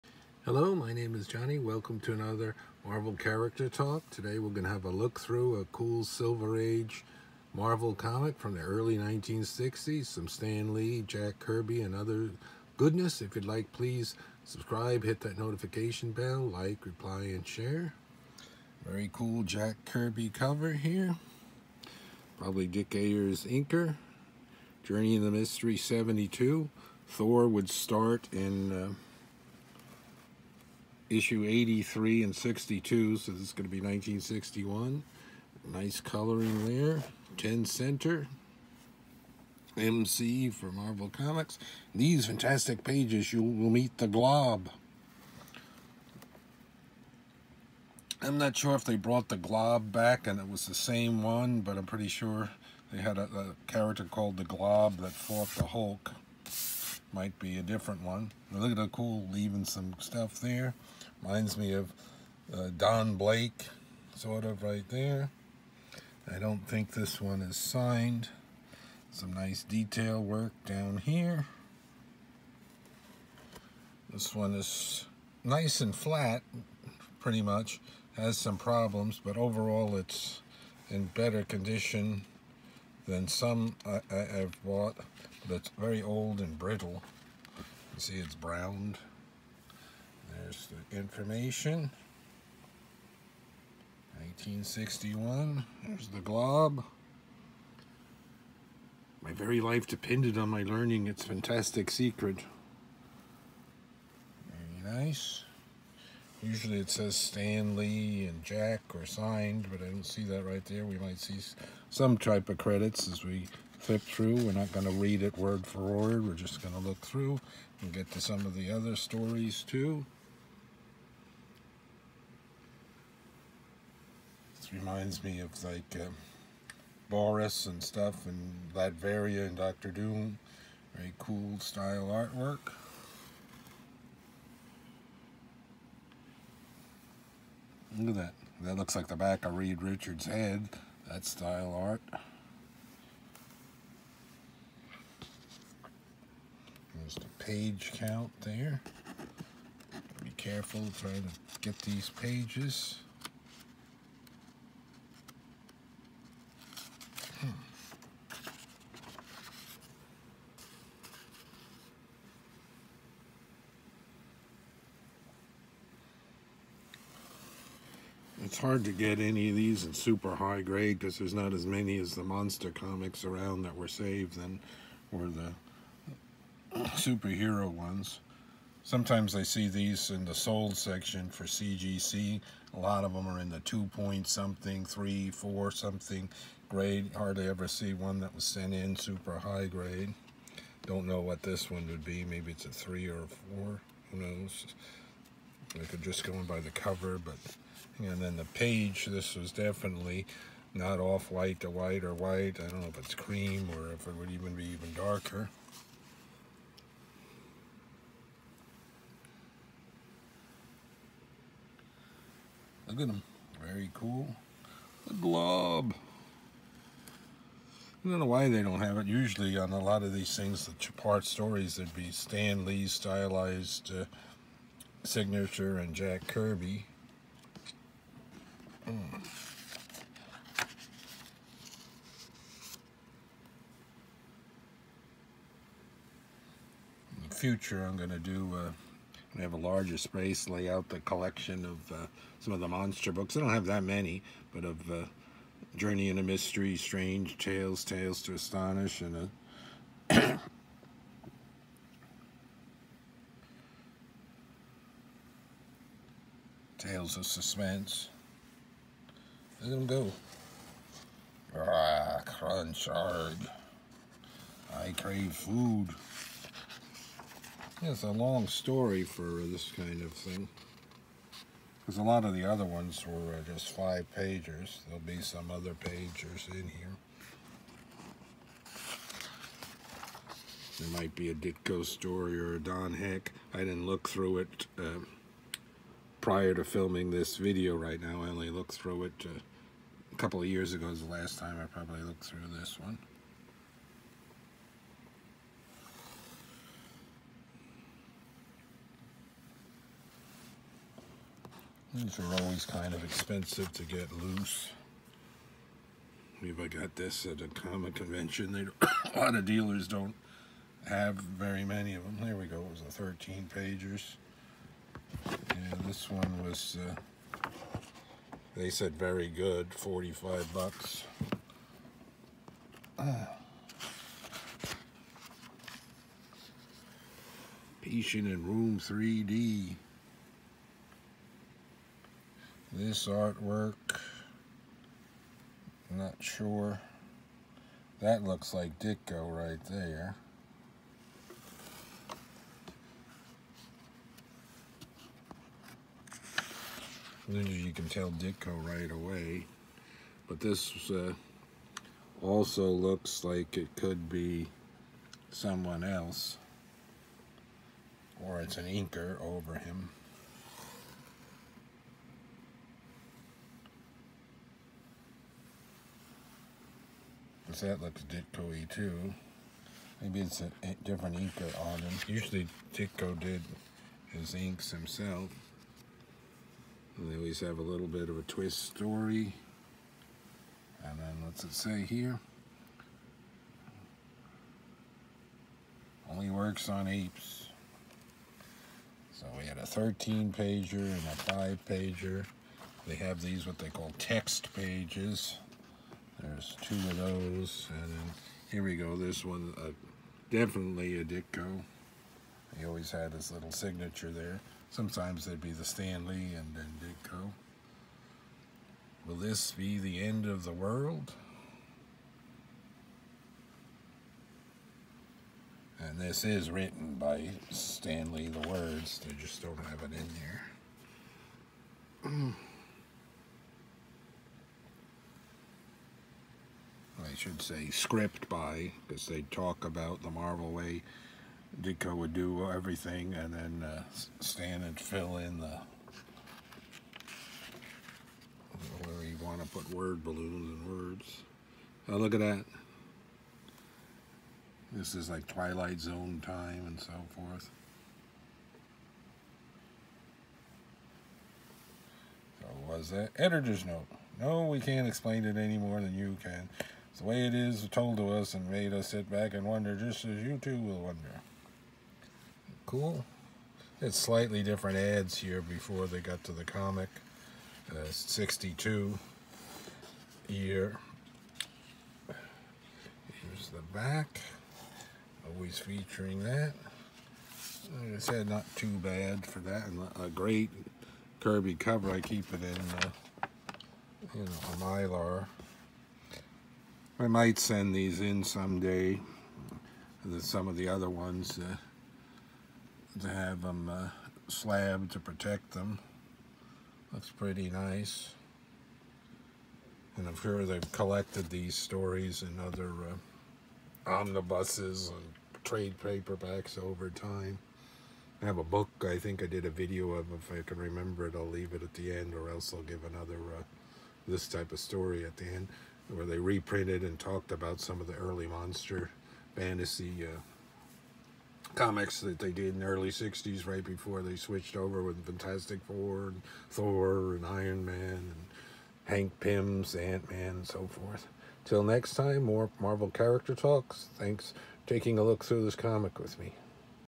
🧟‍♂️💥 Marvel Monsters Glob Journey Into Mystery 72 1961 Marvel Comics look thru issue Jack Kirby🔥📚
🧟‍♂️📚💥🔥 Marvel Character Talk : JourneyIntoMystery 1961 issue # 72 Marvel Comics A look through the issue, art by and cover Jack Kirby. Marvel Silver Age Monsters Glob 💥Silveragecomics🔥Stan Lee, Jack Kirby💥Part of the AwesomeCollection of Marvel Comics and Collectibles.